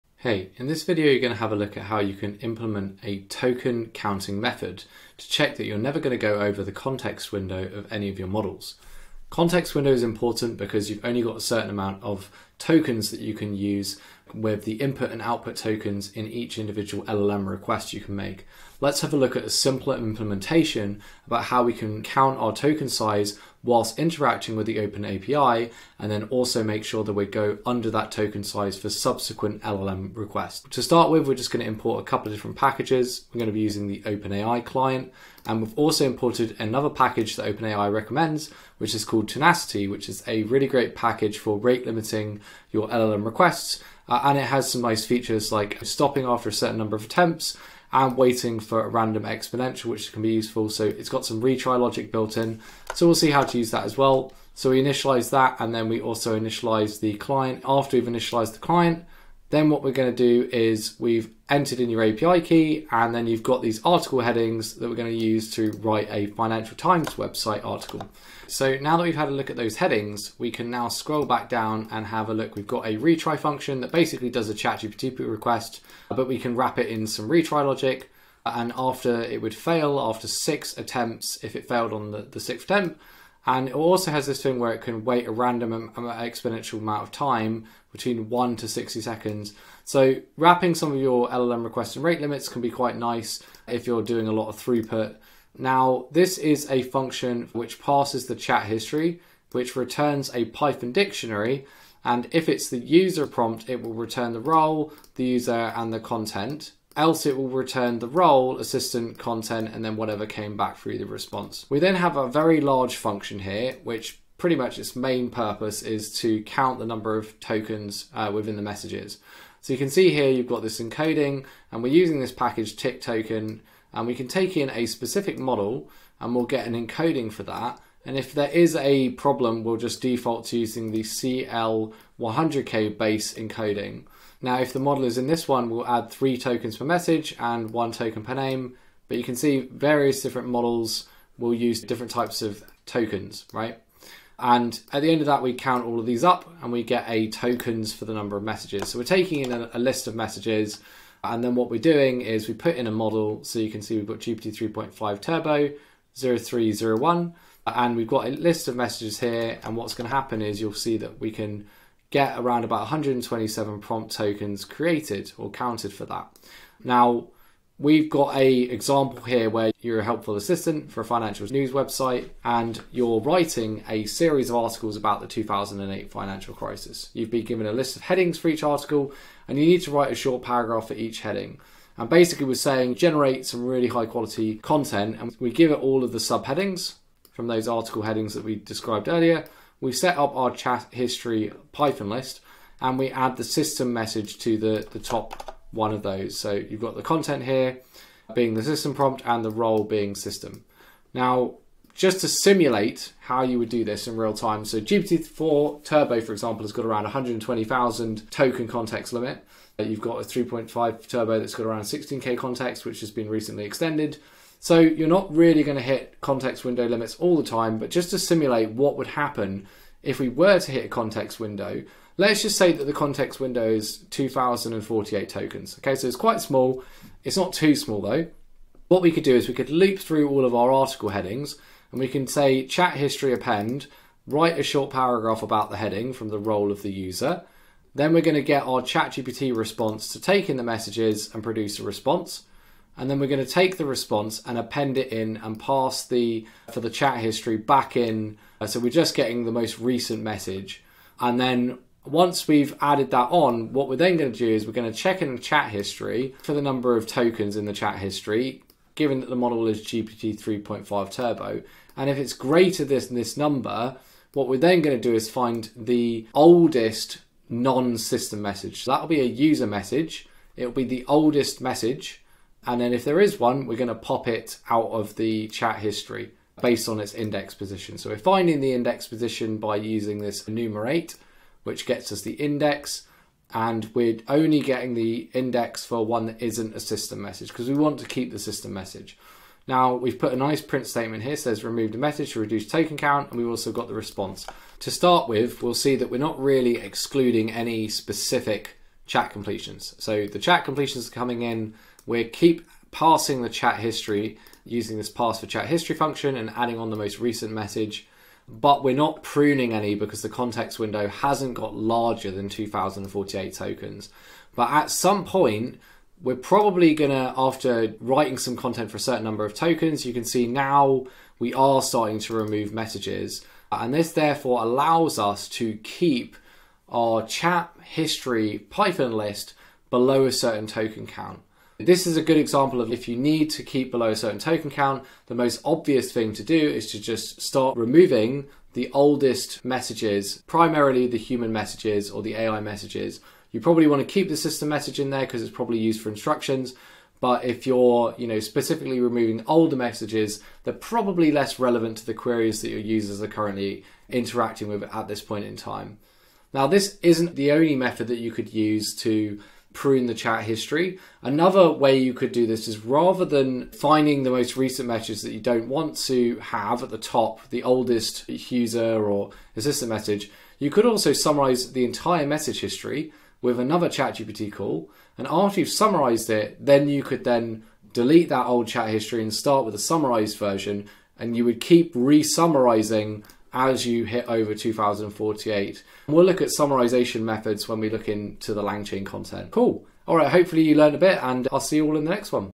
Get Better Outputs with LangChain and Llama3 | AI Prompt Engineering
What if I told you that the secret to mastering AI isn't in the code, but in the questions you ask?

In a world where technology evolves at breakneck speed, the ability to prompt effectively can be the difference between success and obsolescence. As a business leader or a curious mind, you need to understand the nuances of interacting with generative AI.

Did you know that mastering prompt engineering can exponentially enhance your AI outputs? Imagine leveraging tools like LangChain and Llama3 to overcome token limits and generate precise, context-aware content. Unleash the power of well-crafted prompts and watch your AI transform from a tool into a true collaborator.